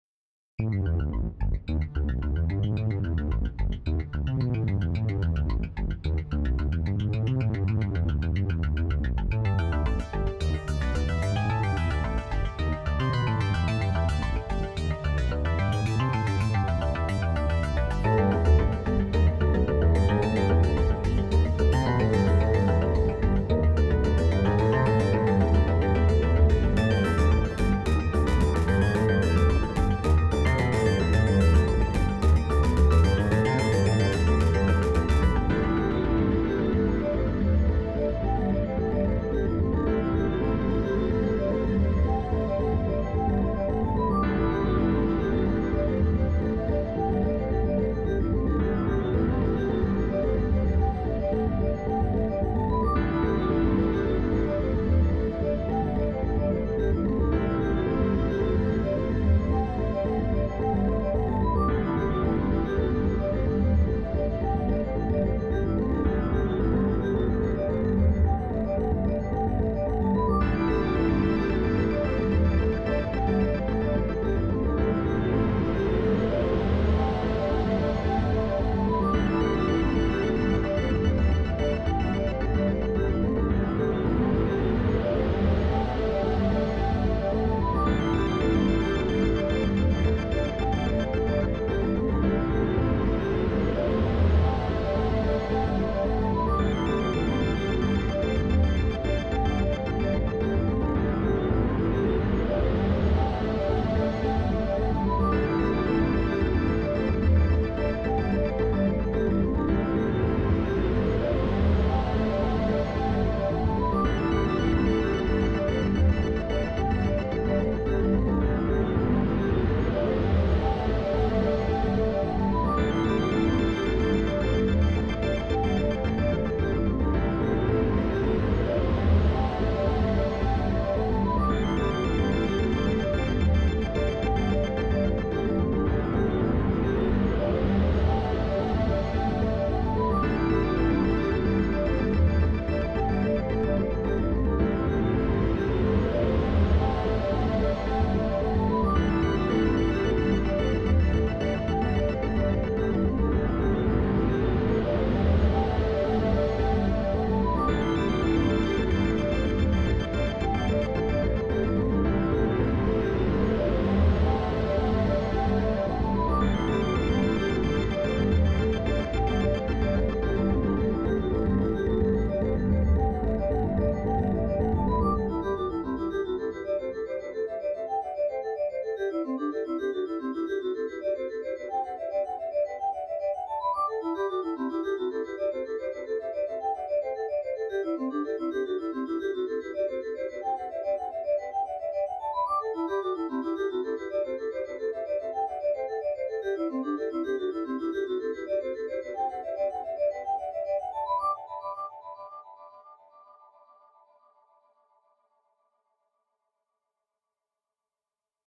Ambient Sequences project 7 - Royalty Free Music
This is a 2 part sequence with plenty of steps in melodic value as well as repetition to frame a situation for your YouTube project. Piano and multiple synths.